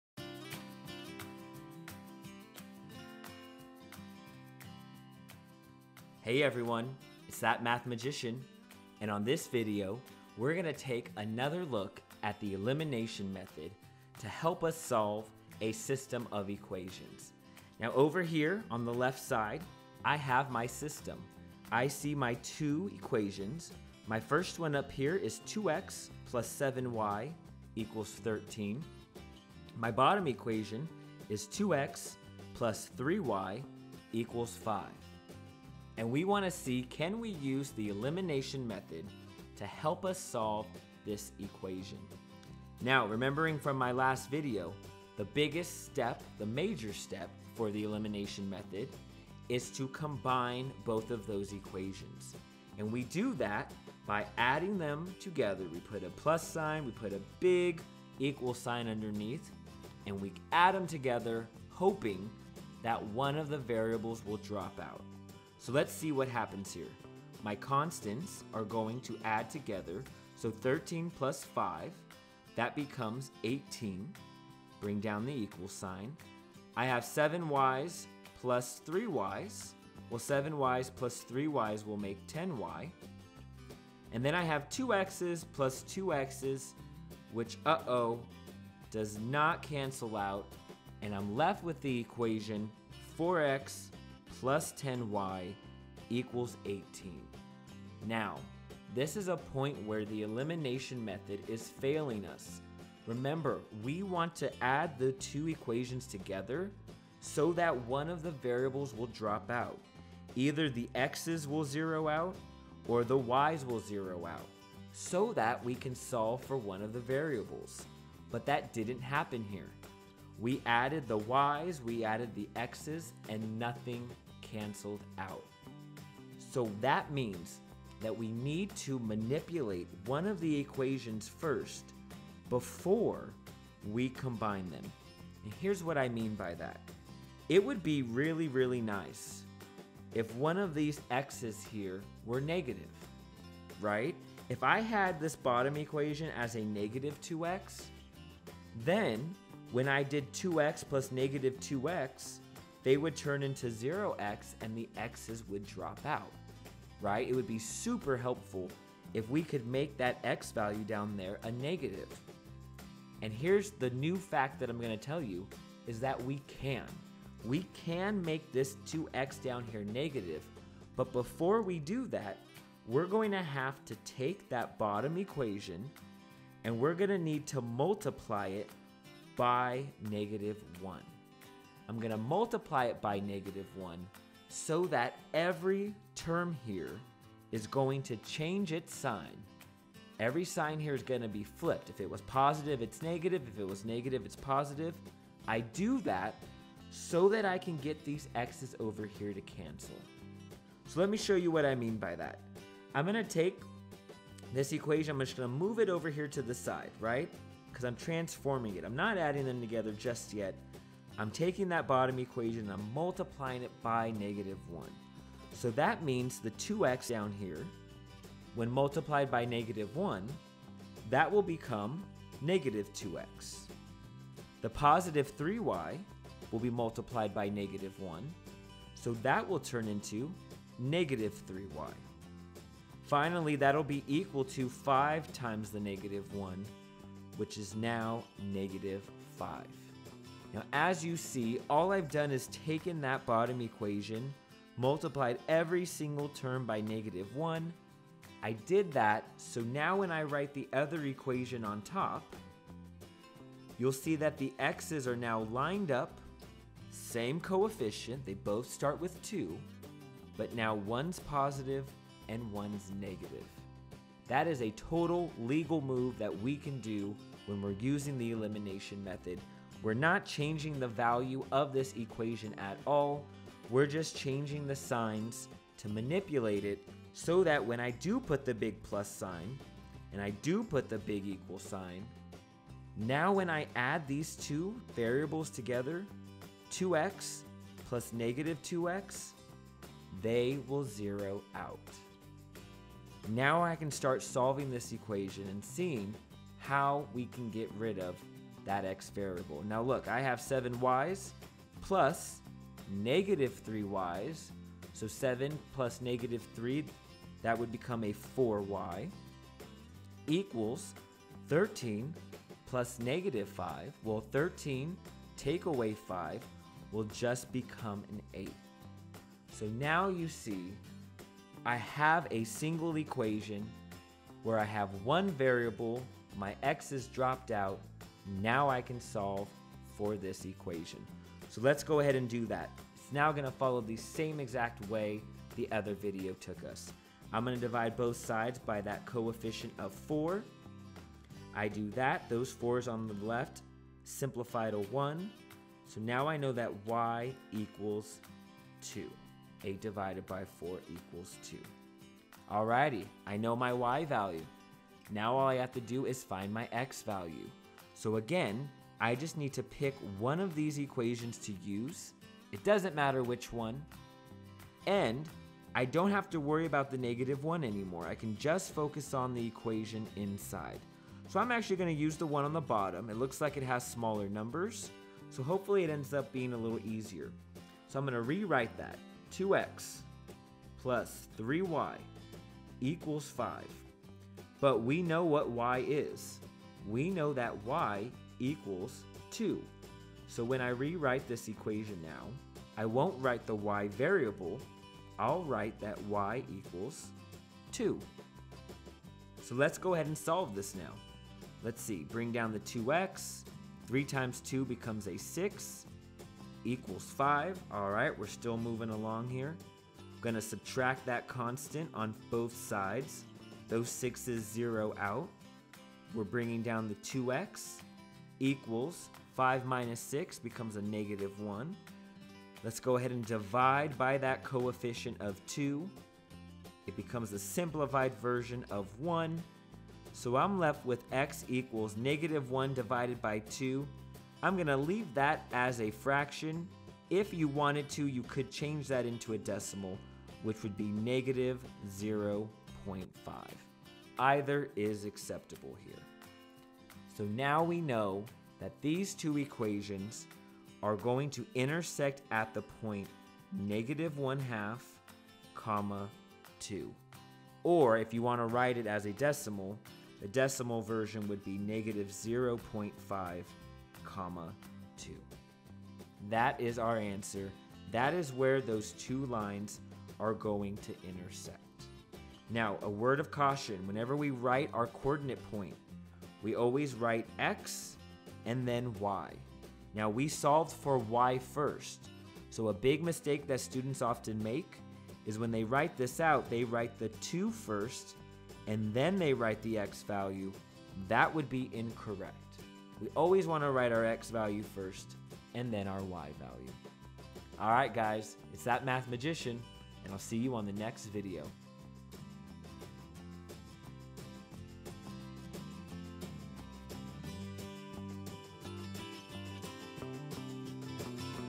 System of Equations: More Elimination Method
Let's look at another example on how we can use the Elimination Method to solve a system of equations!